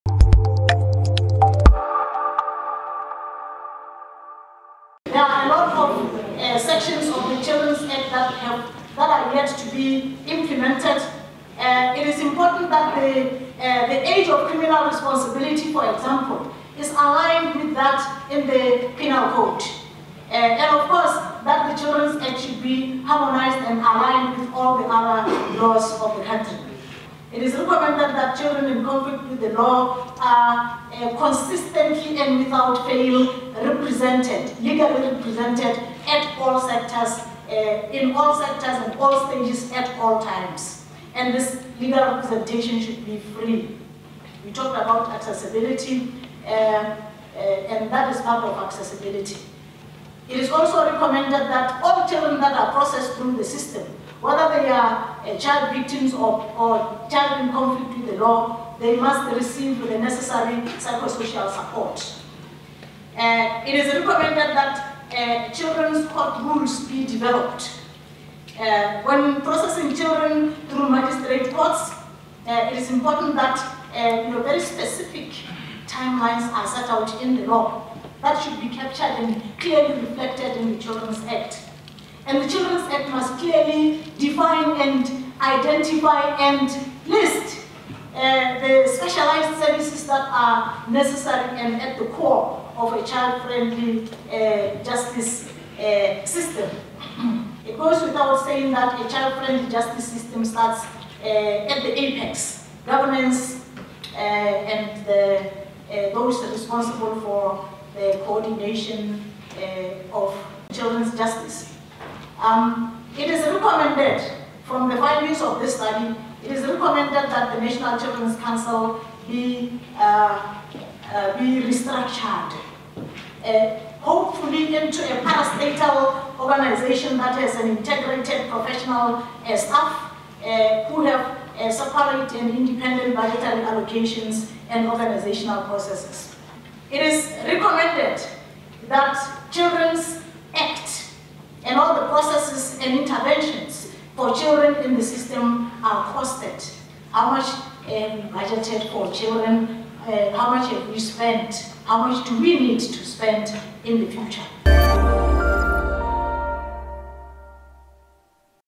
UNICEF
WATCH: Researchers at UNICEF have found that the Children's Act is poorly implemented and has many loopholes that need to be addressed. They have also recommended abolition of corporal punishment in schools, and for a justice system that is friendly to children.